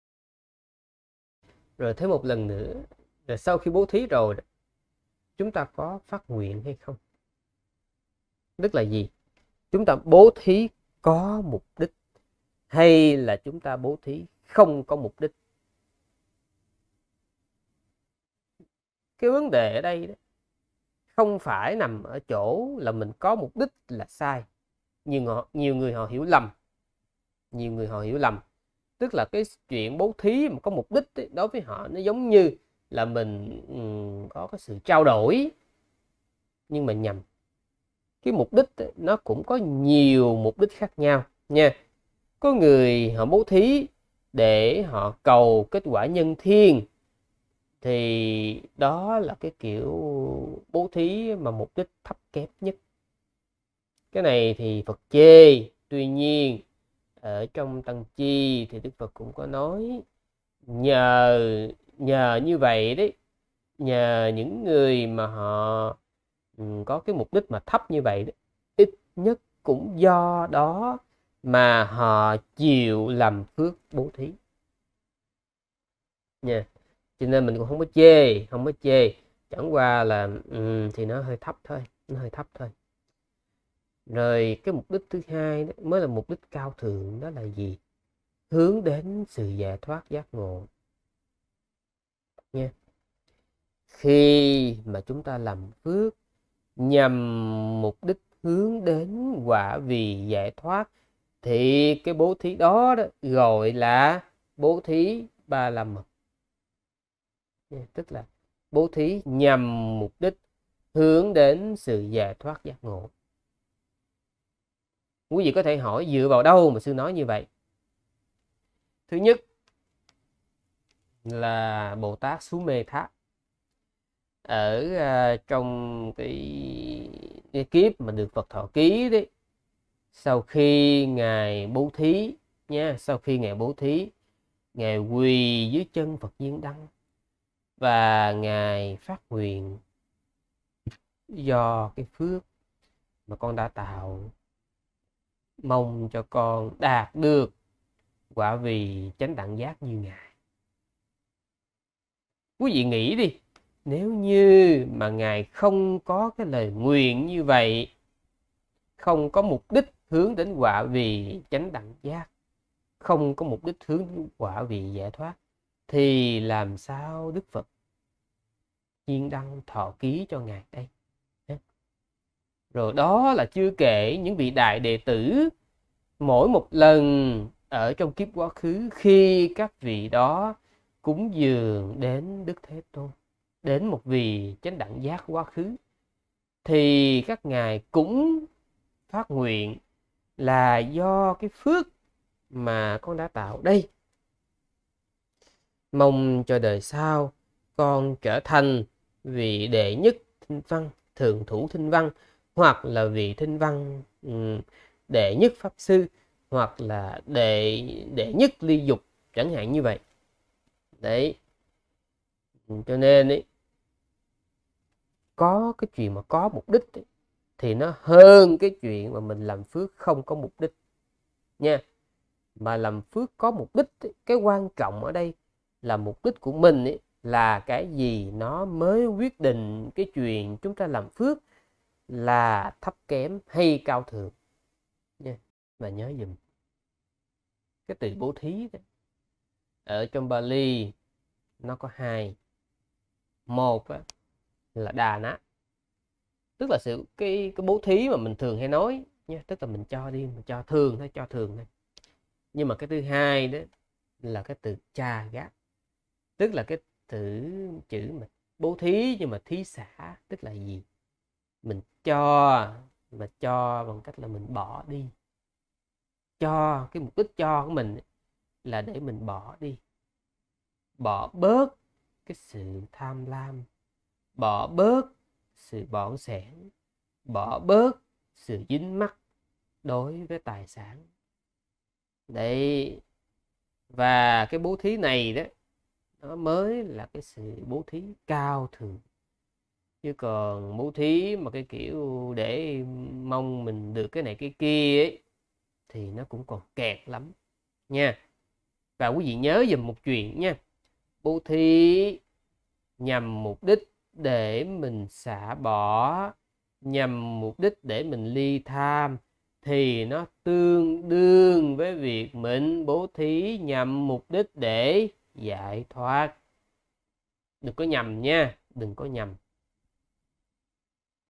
Bố thí có mục đích | Sư Hạnh Tuệ
Trích trong bài giảng Hiểu về bố thí Lớp Phật Pháp Căn Bản